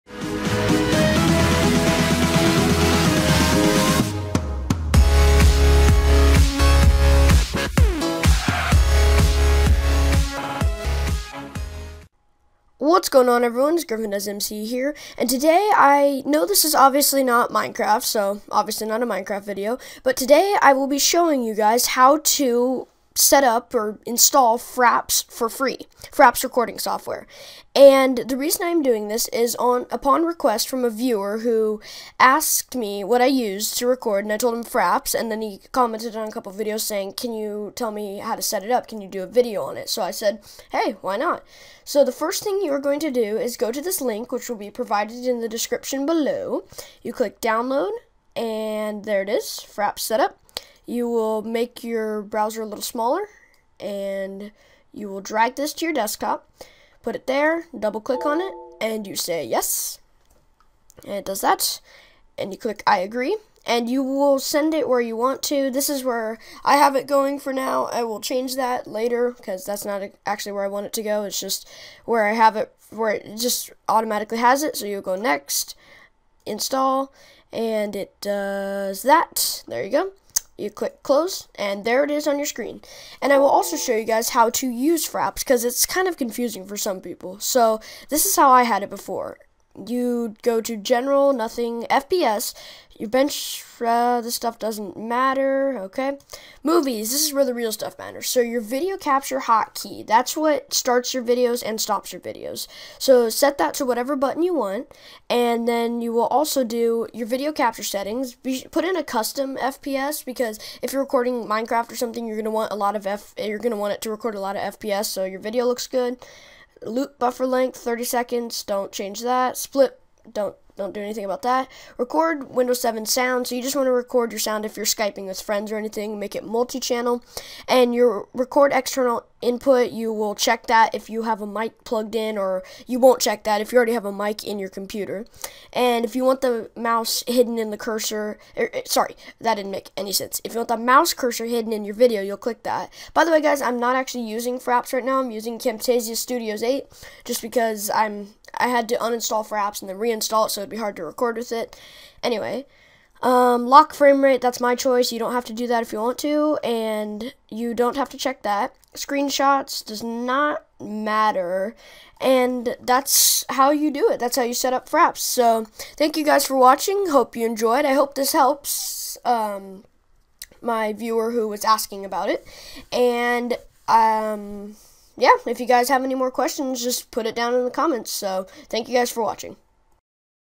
Fraps "Cracked" Installation and turtorial
Hey guys I'm here with a tutorial video. In this video I am showing you guys how to download and install Fraps recording software. I hope this helps ClimbingMonkeyMC.